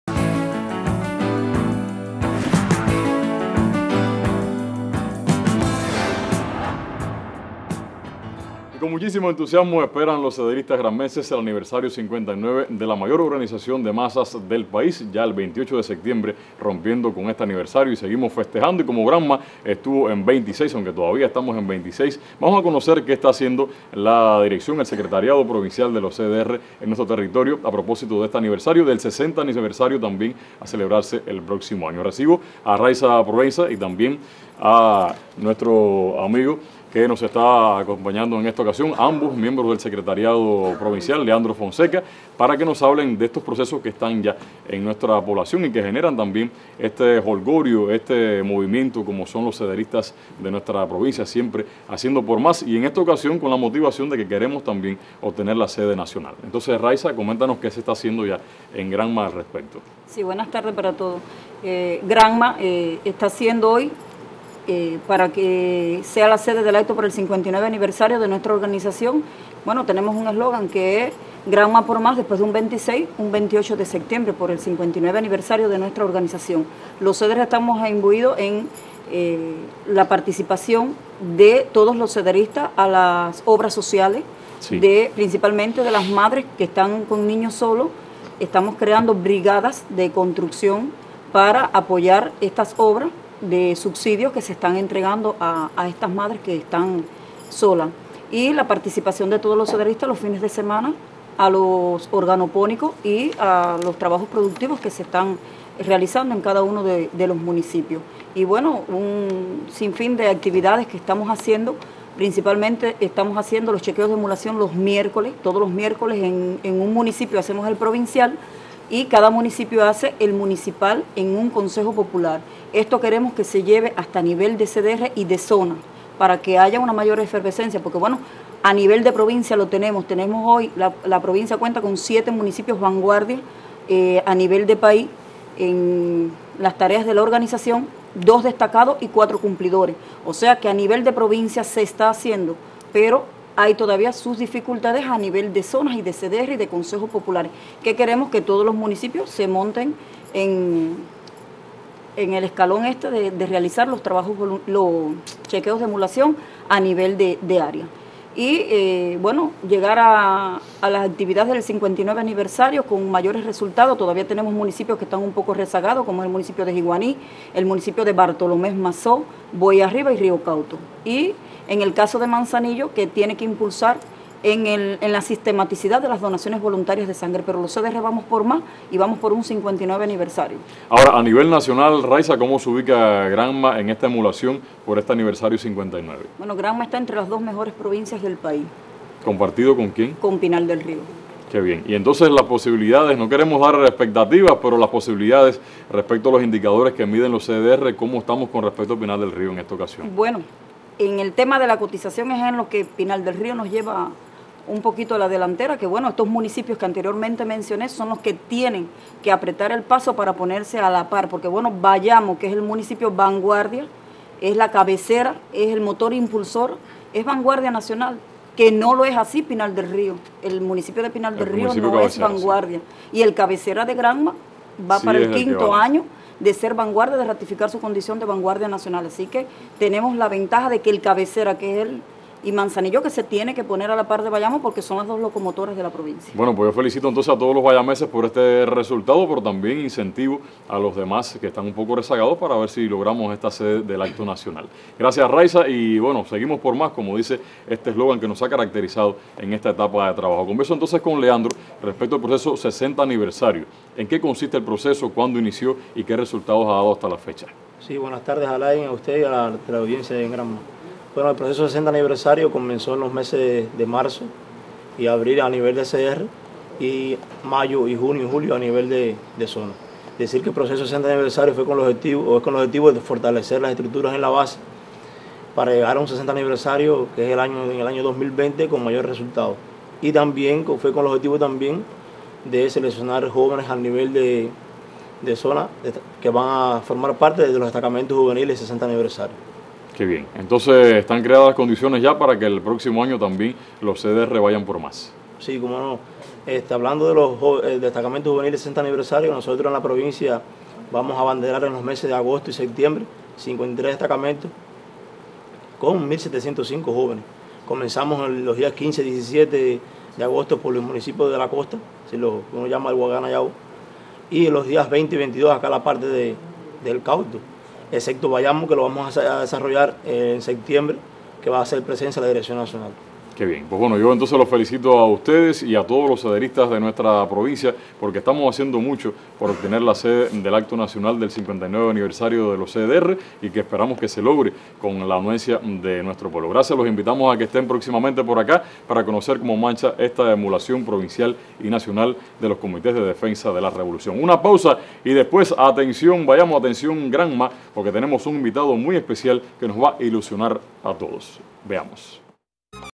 Cederistas granmenses trabajan por mejores resultados
La mayor organización de masas del país trabaja por mejores resultados. En Granma, los CDR se mantienen en continua labor.